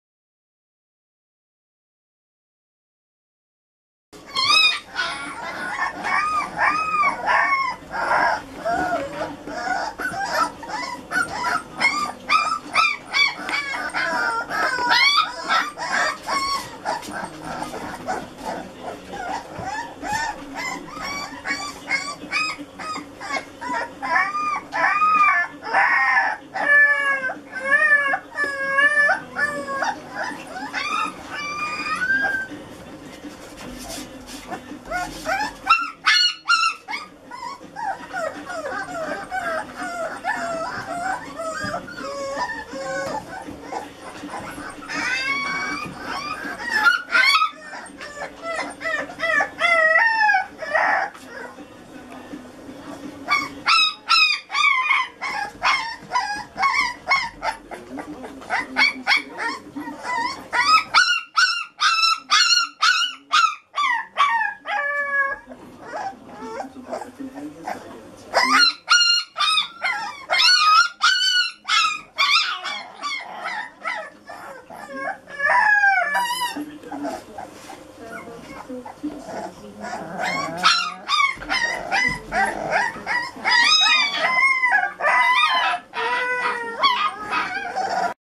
Eldsjälens NHL-kull ylar 4 veckor
Eldsjälens NHL-kull ylar i valplådan 4 veckor gamla